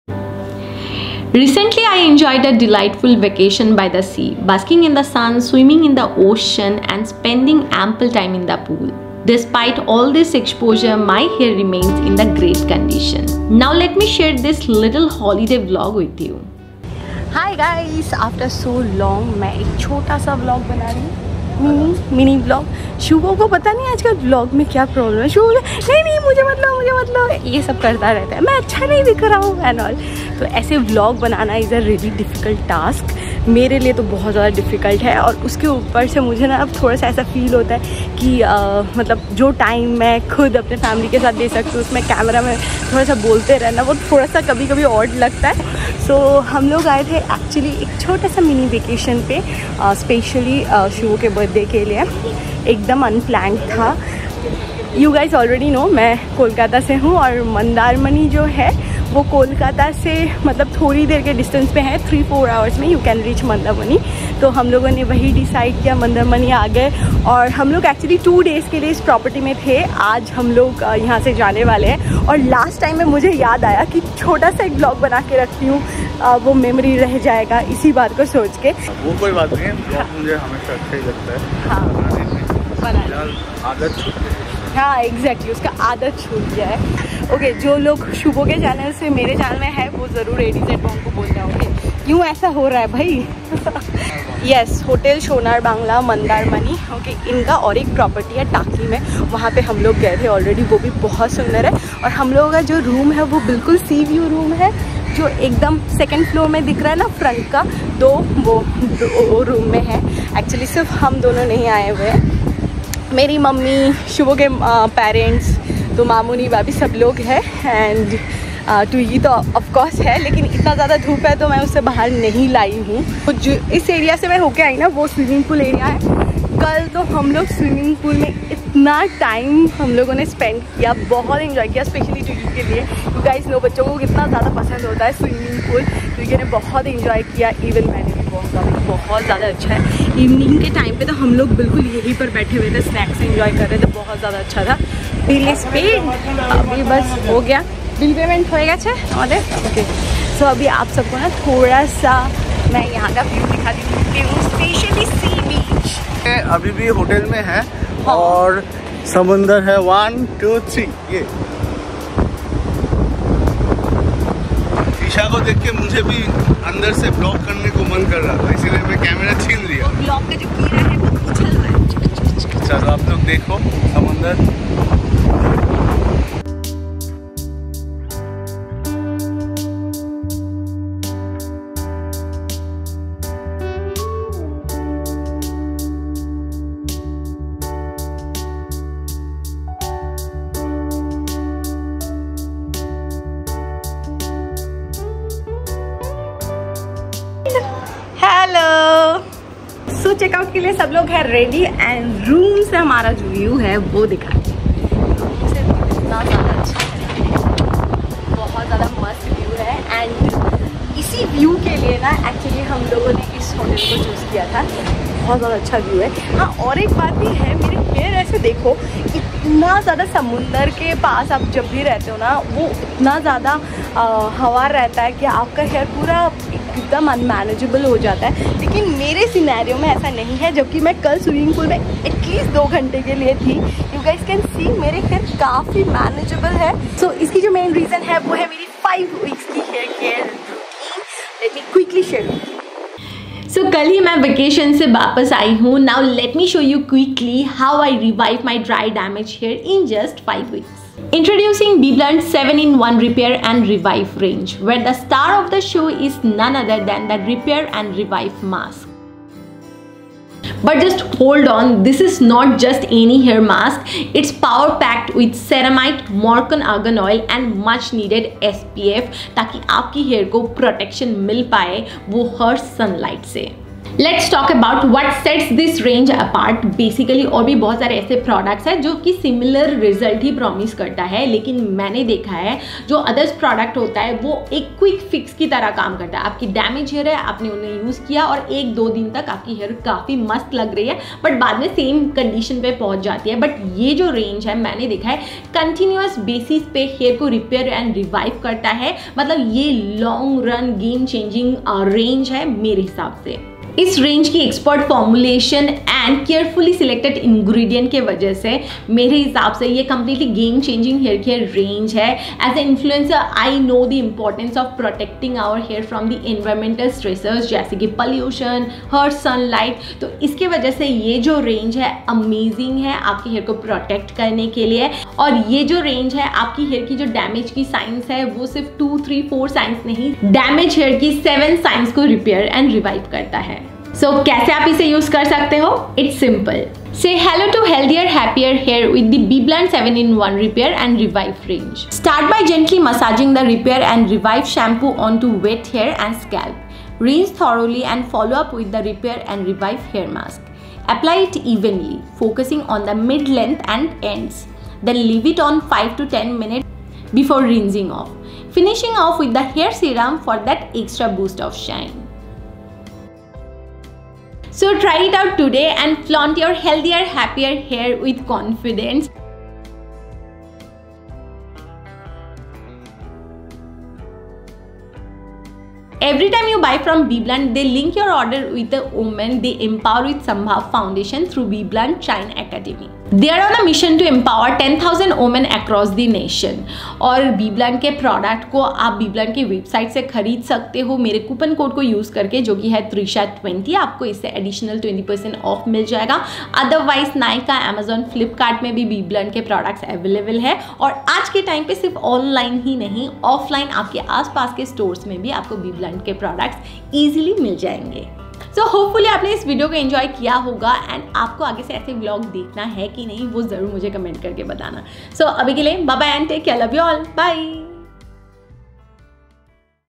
Family beach Vacation ☀️🏖️ sun, Sand, and Smiles.
Join me on a relaxing beach vacation as I soak up the sun, explore beautiful coastal spots, and share my beach day essentials! 🏖️ Plus, I’ll let you in on my secret to keeping my hair smooth, frizz-free, and manageable despite the salty air – the Bblunt hair care range. 🌟 Watch till the end for my full hair care routine and tips! 💫

Products Mentioned